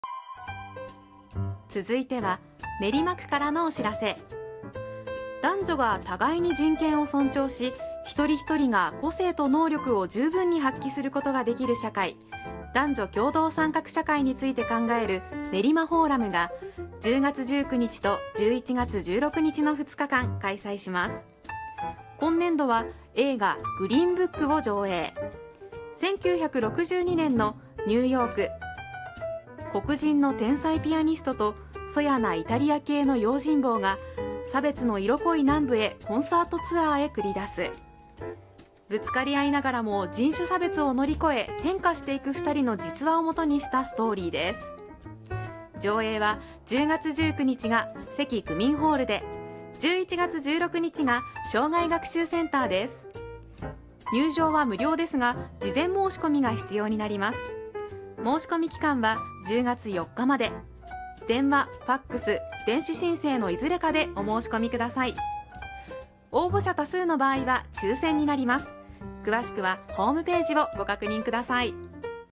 ネリマフォーラム フィルムフェスタ 2024
映画「グリーンブック」上映会
10月19日（土）関区民ホール
11月16日（土）生涯学習センター
★事前申込制。応募者多数の場合は抽選。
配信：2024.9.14